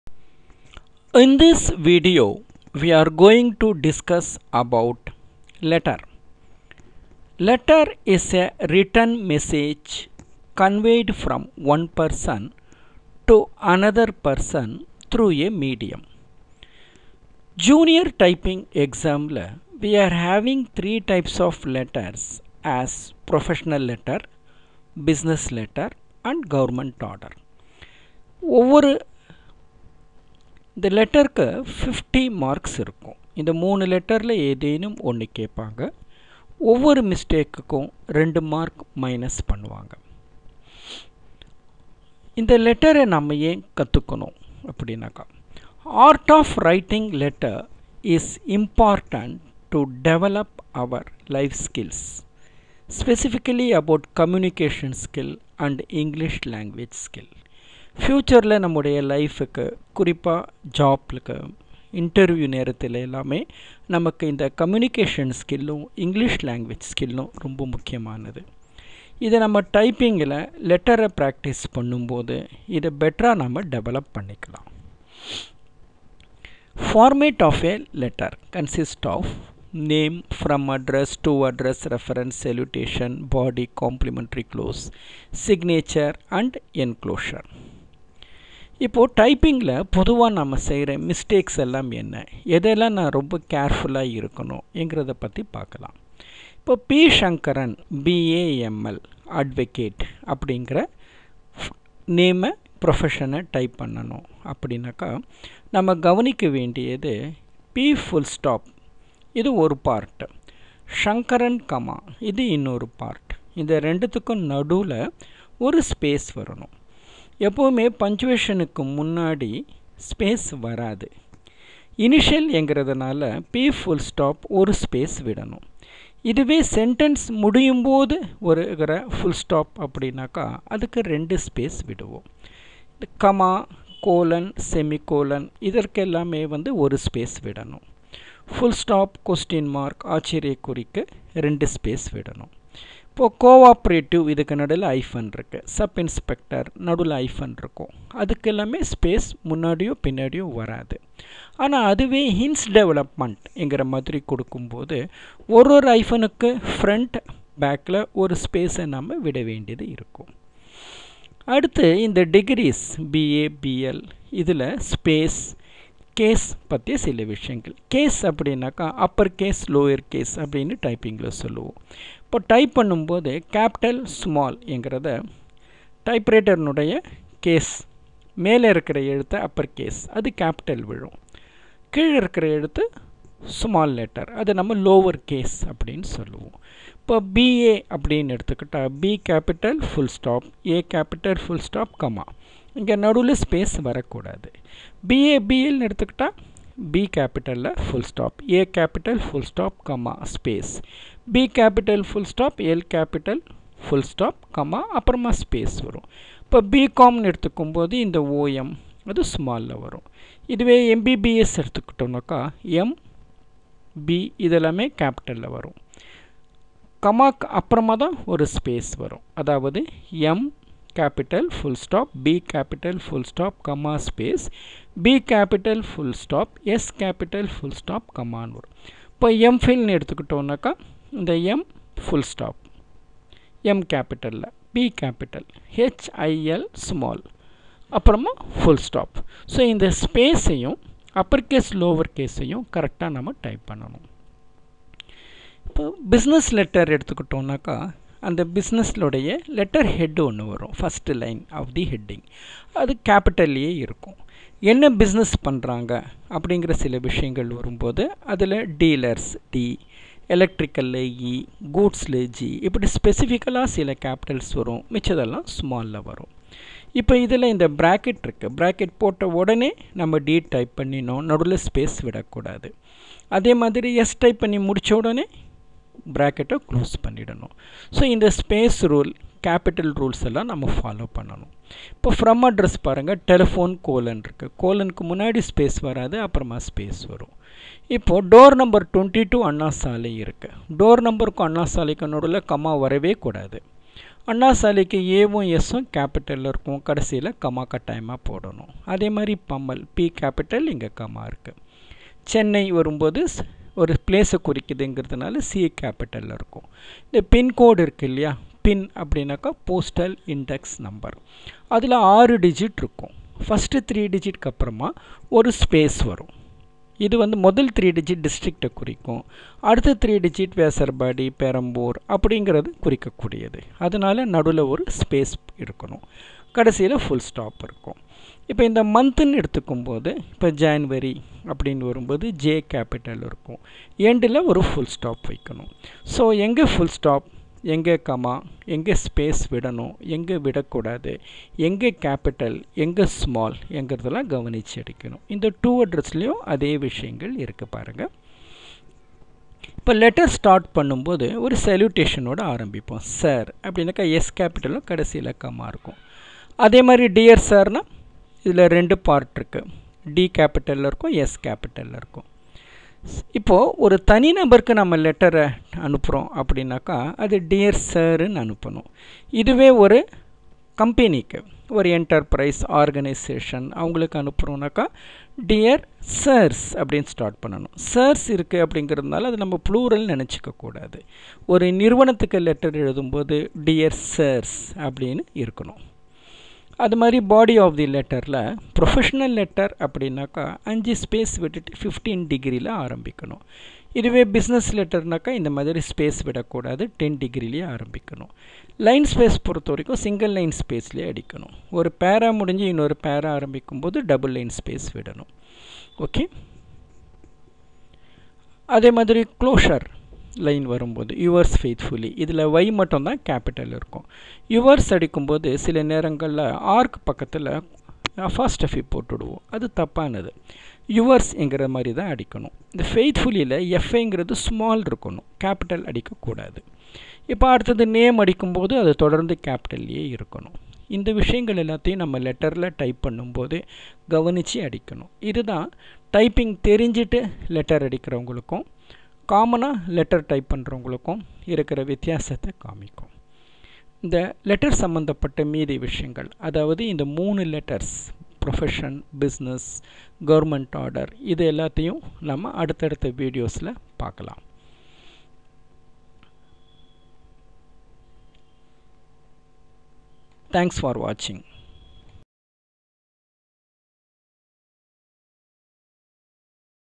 INTRODUCTION TO LETTER
It is prepared by Vajid Sha S.  It is useful for typing students specifically for junior typing exam students.  It is useful to develop art of letter writing.